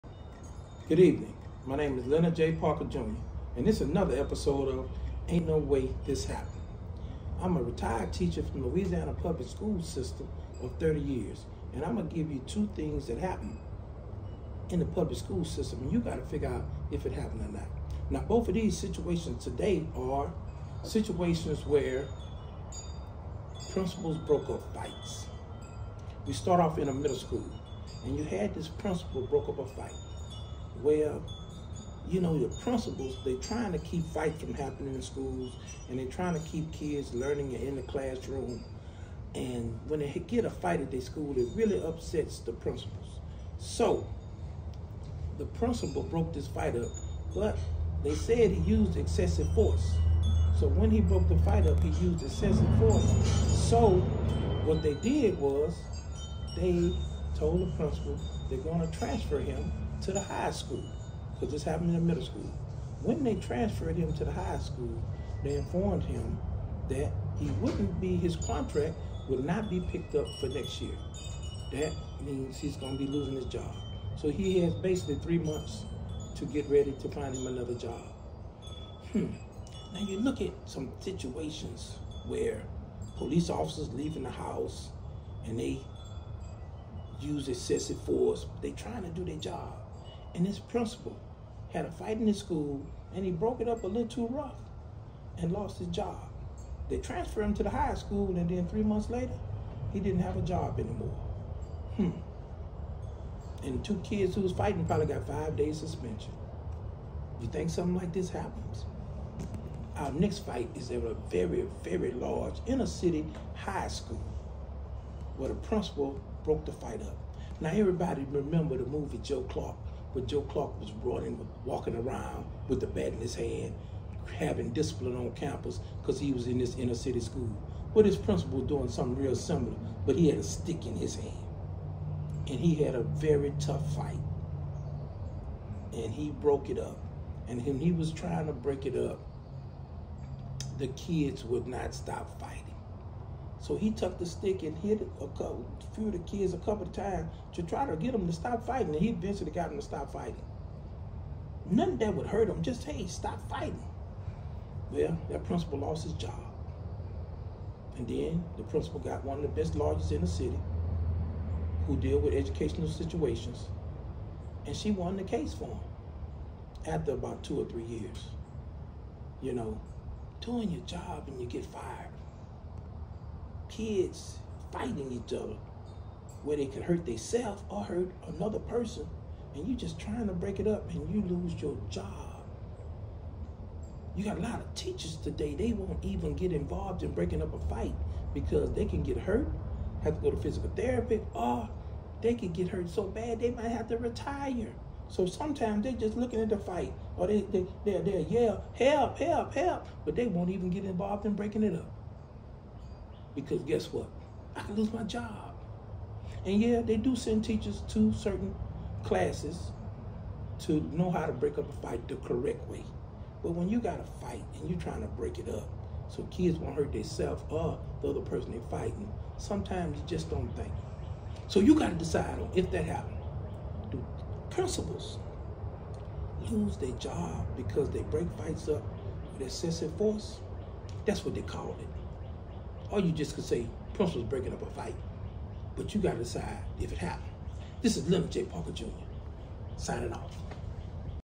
Ain’t no way this happened (Middle school and High school principal breaking up fights)
30 year retired teacher tells stories that happened in Louisiana public schools